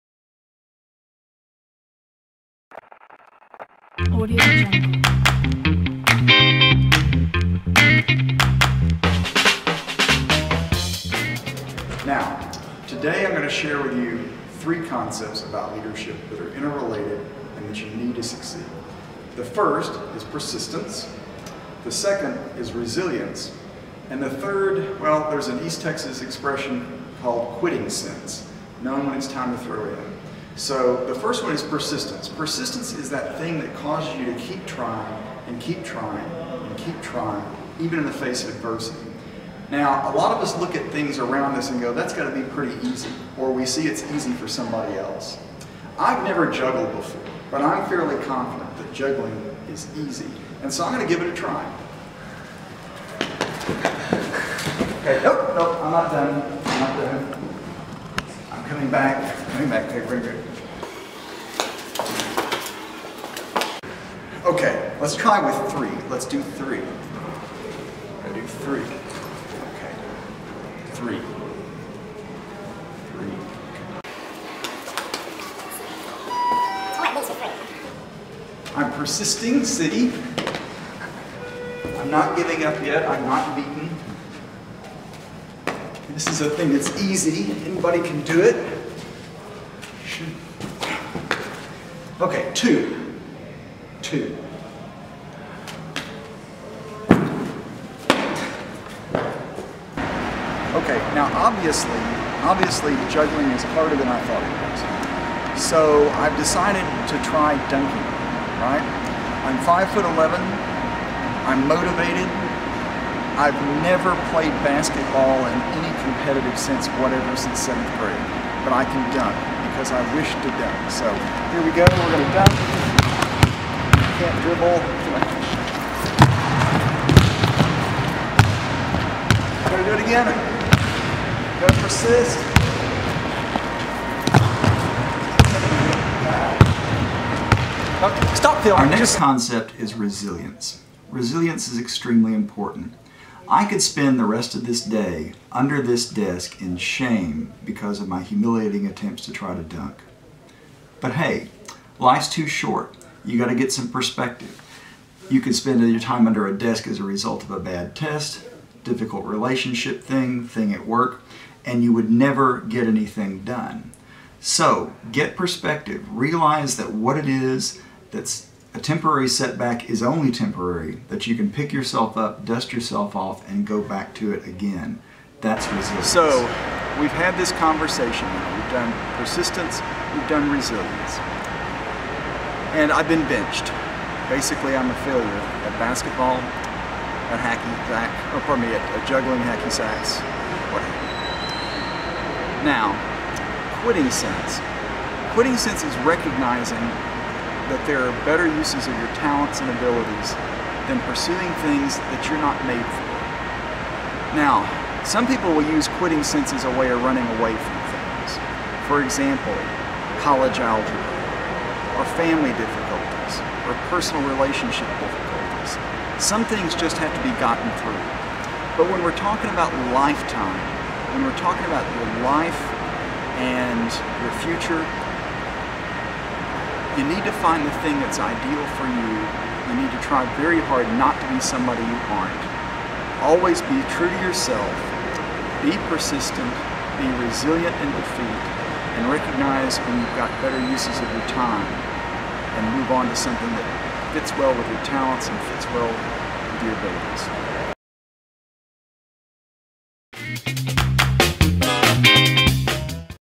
Leadership Tip(s) of the Week - Ricky Dobbs
Dr. Ricky Dobbs is the Dean of University College. In this entertaining video compilation, he explains his three-prongred approach to Leadership: Persistence, Resilience and Quitting-Sense. Be prepared to watch Dr. Dobbs attempt to conquer juggling hacky-sacks, strive to dunk a basketball and explain a point underneath a table. Needless to say, you see a side of Dean Dobbs that very few get to see on a daily basis.